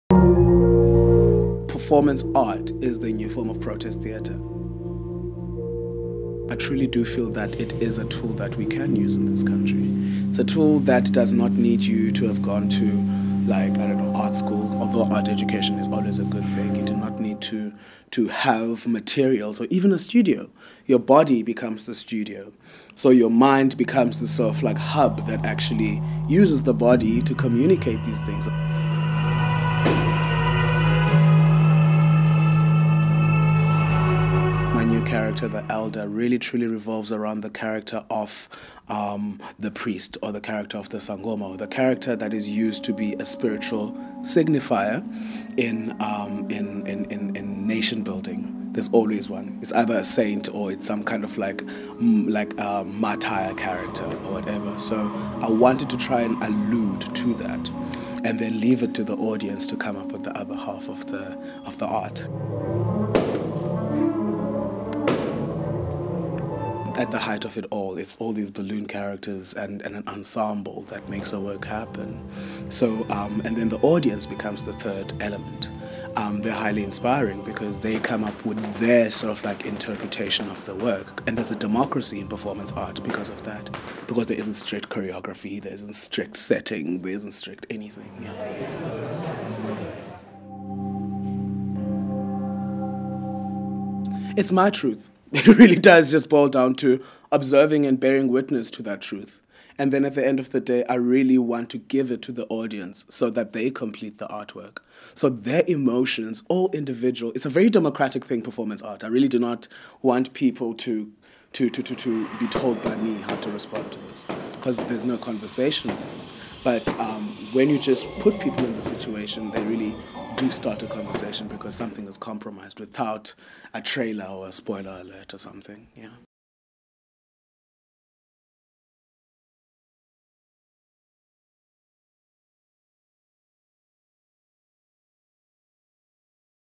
Athi Patra Ruga - Standard Bank Young Artist (Performance Art)
Athi-Patra Ruga is winners of this year’s Standard Bank Young Artists Award for Performance Art.  He talks about the award and his the premiere of his character the Elder of Azania at the National Arts Festival 2015.

Report by Rhea MacDonald and Teboho Ramosili